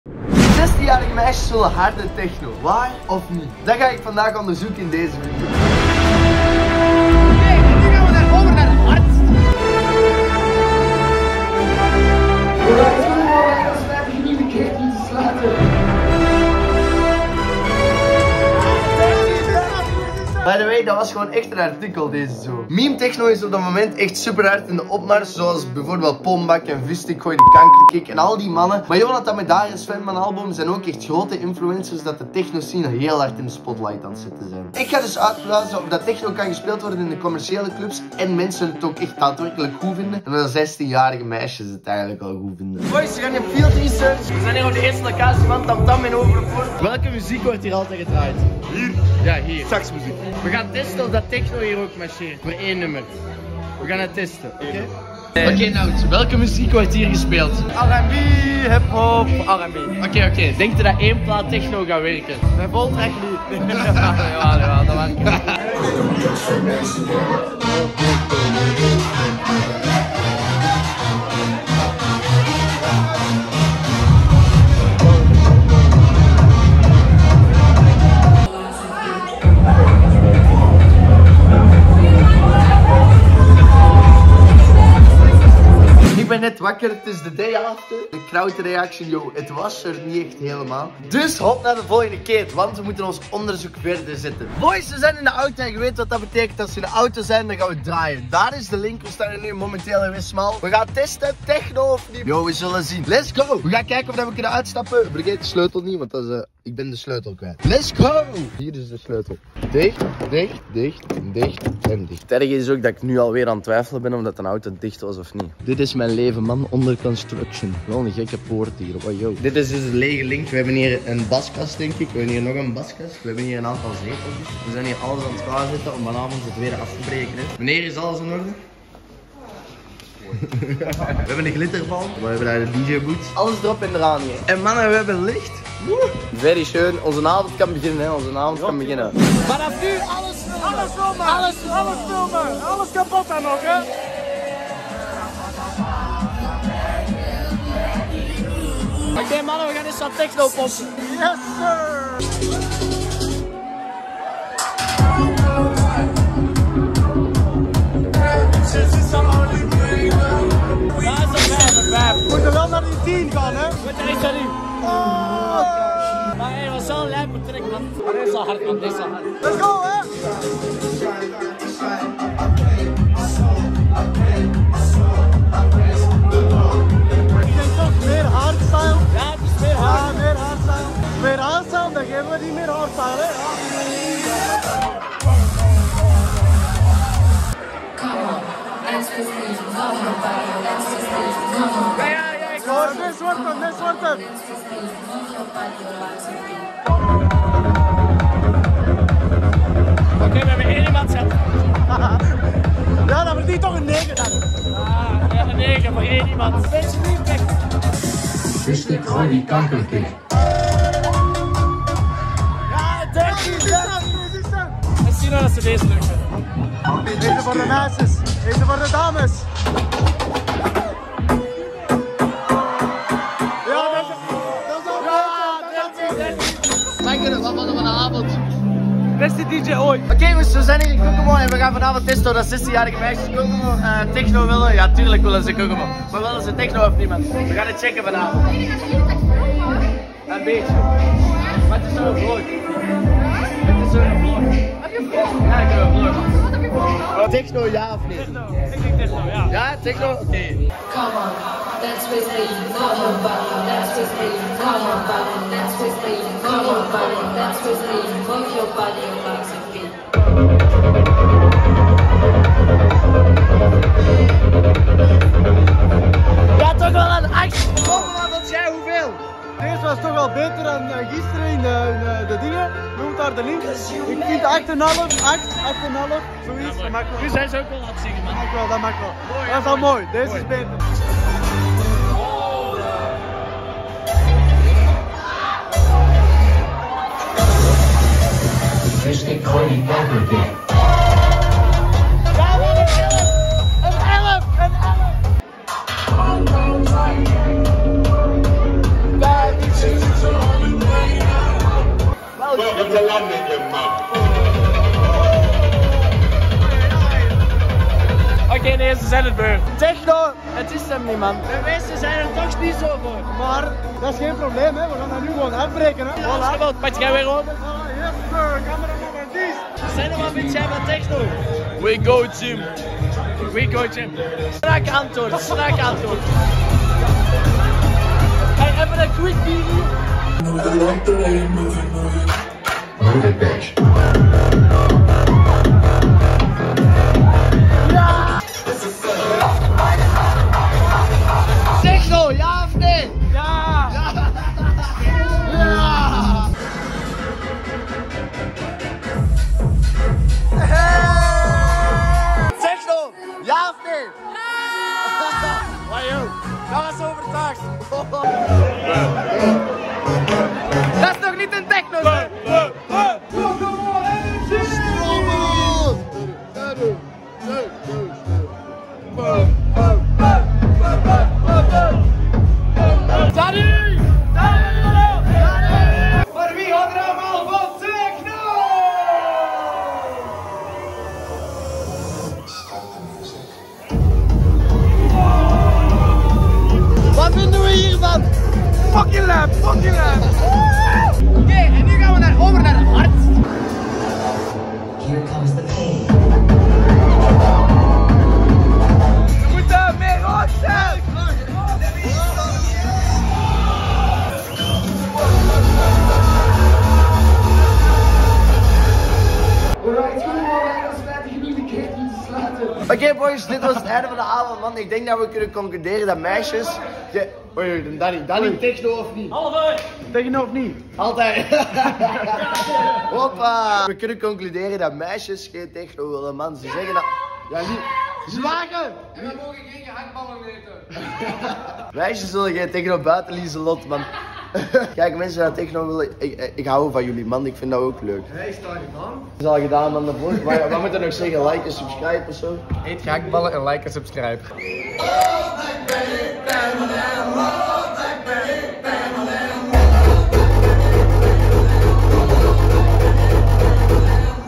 WILLEN 16-JARIGE MEISJES HARDE TECHNO?
techno dj vlogger YOO GUYS!!! Vandaag ging ik testen ofdat 16-jarige meisjes weldegelijk harde techno nice vinden? BLIJKBAAR ZIJN DE MENINGEN VERDEELT / ANYWAY LAAT ME WETEN WAT JE VOND VAN DE VIDEO IN DE COMMENTS YEEHAAA

Check ook zeker mijn socials & playlists! :D